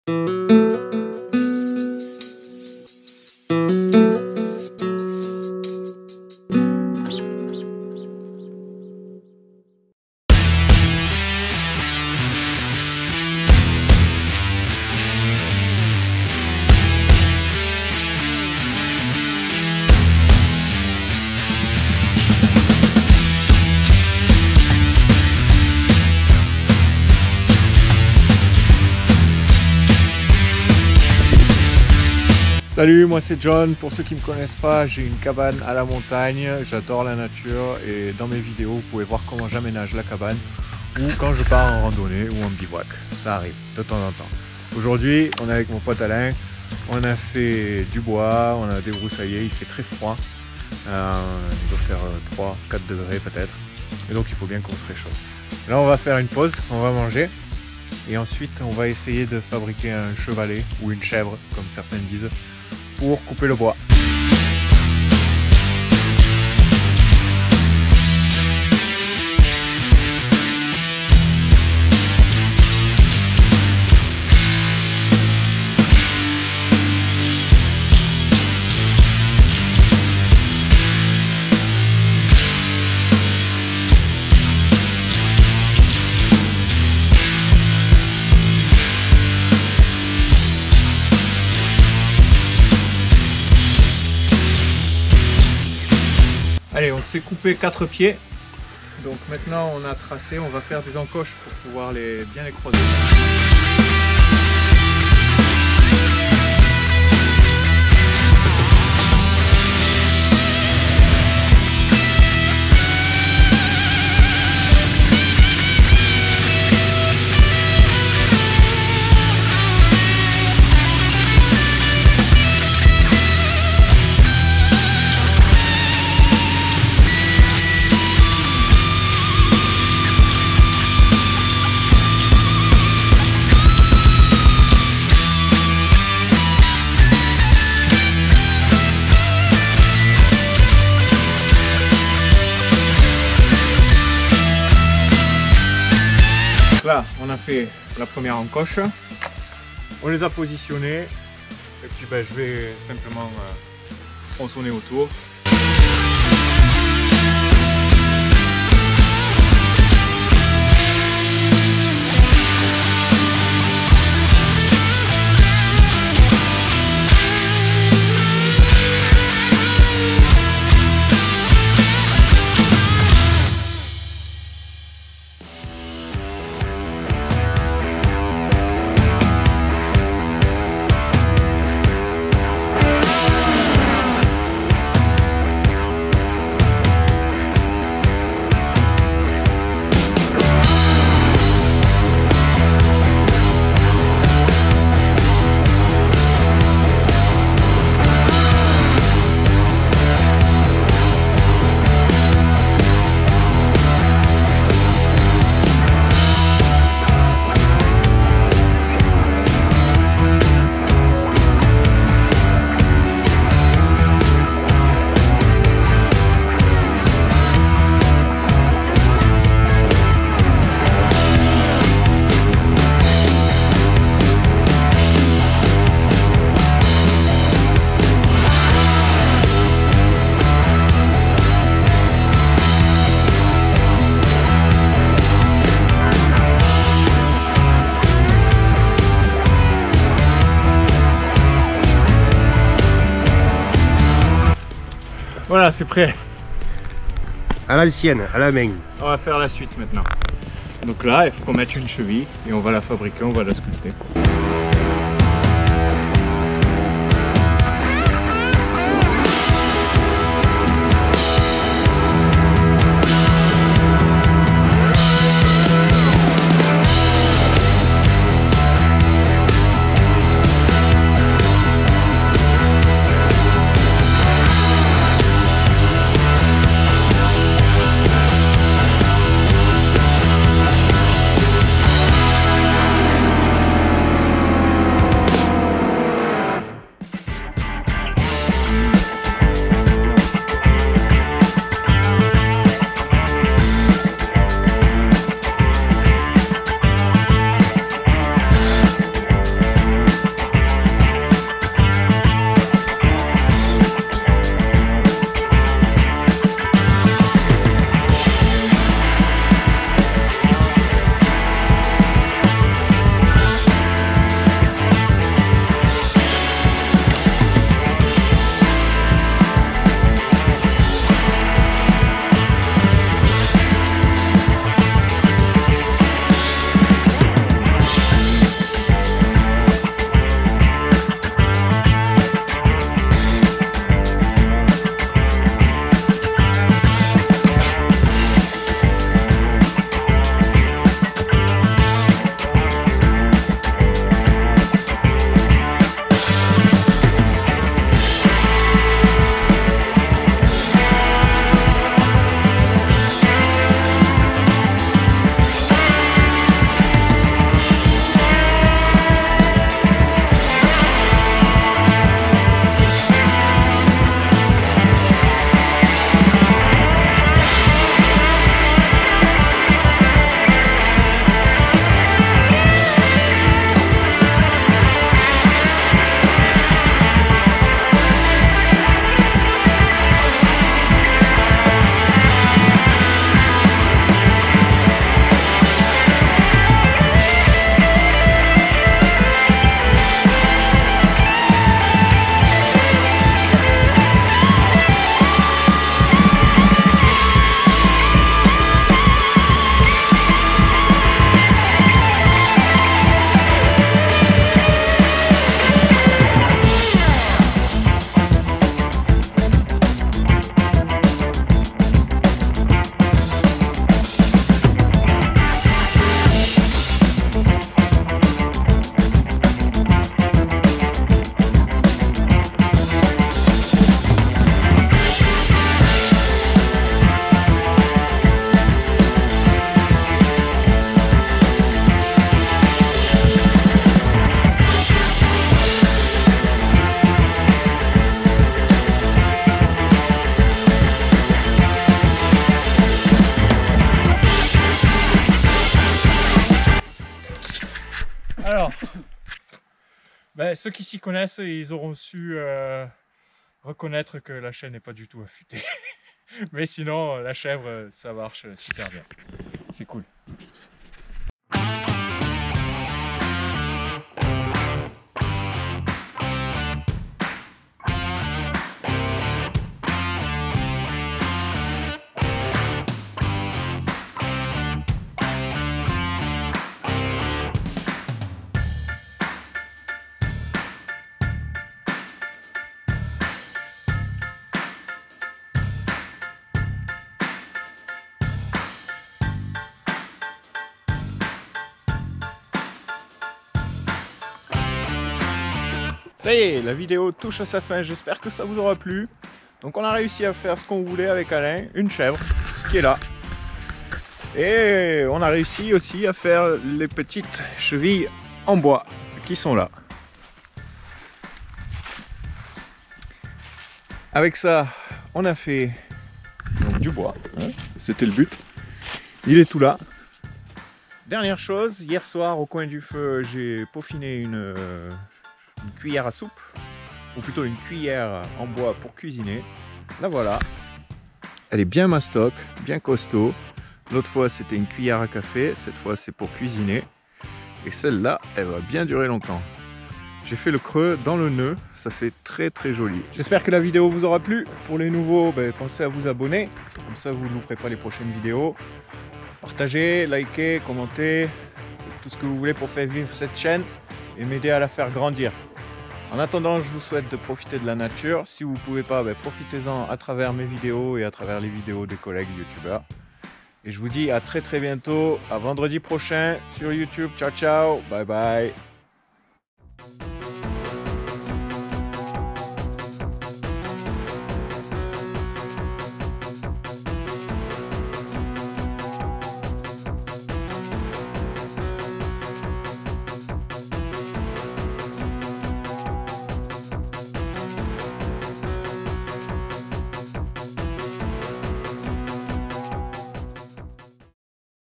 Fabriquer Un Chevalet De Tronçonnage
Dans cette vidéo on vous montre comment on a fabriqué un chevalet (souvent appelé chèvre) pour tronçonner le bois de chauffage.